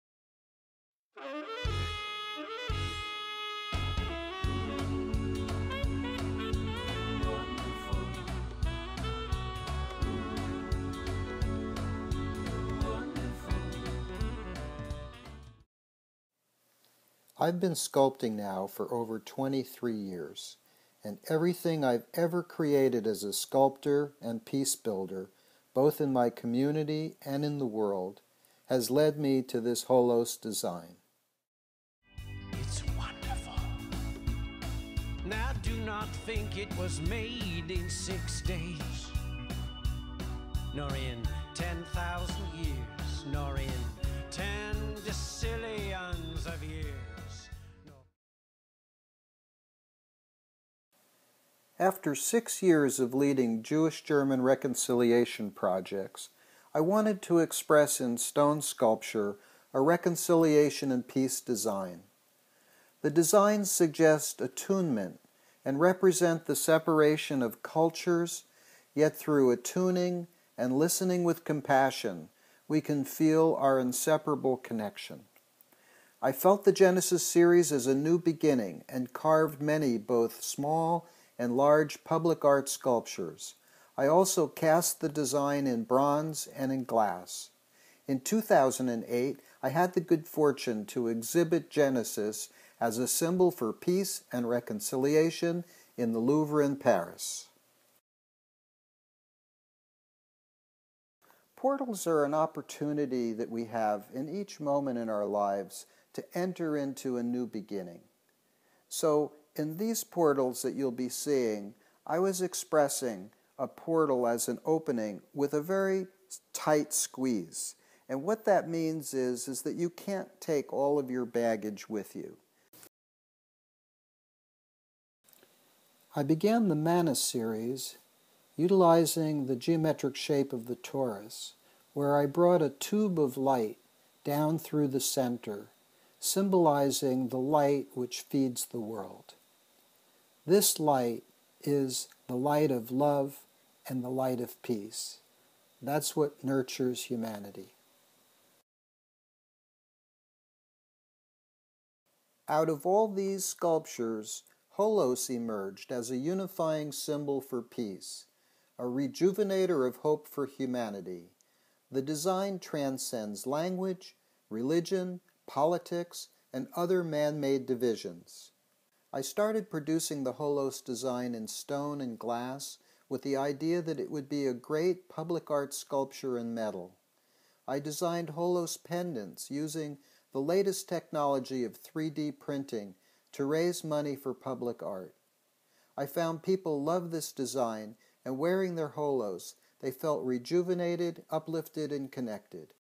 Sculptor as Peace Builder and Artist for Peace
This video gives the origins of what lead up to me creating the HOLOS design, and it symbolizes our One Humanity.